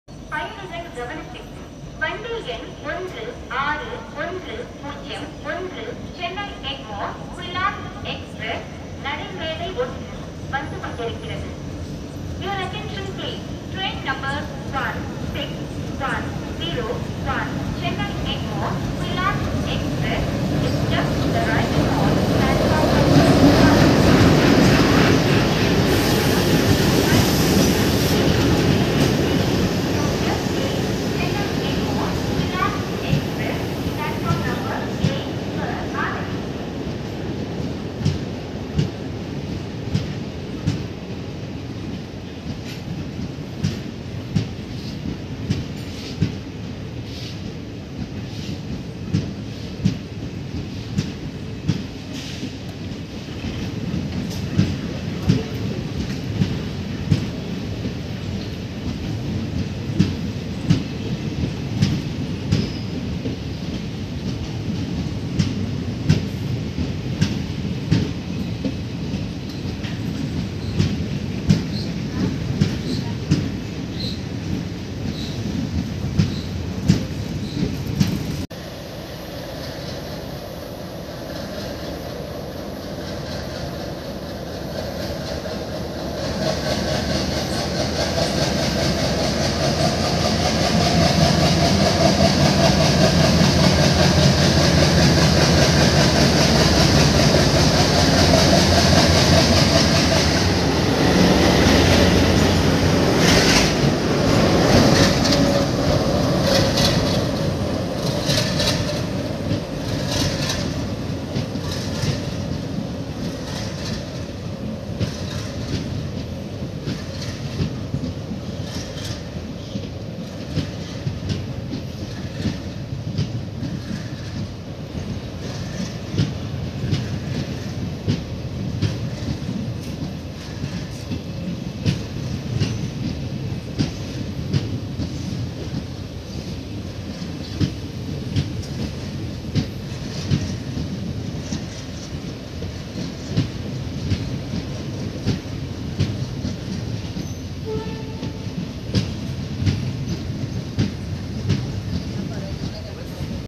Early morning Kollam mail announcement & arrival at Tenkasi Junction
1. GOC WDG3A Chennai Egmore Kollam Junction mail

2. Palakkad Tirunelveli Palaruvi Express